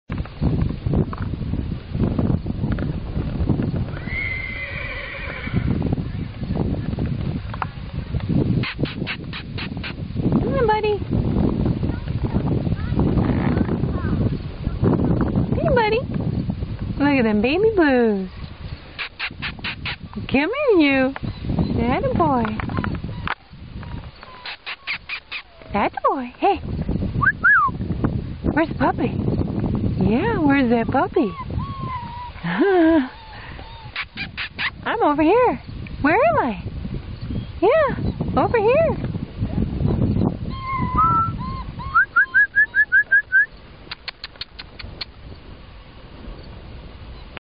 BLUE EYED LILAC MERLE "NEON"
Video of a cute Border Collie puppy for sale. BLUE EYED LILAC MERLE  "NEON"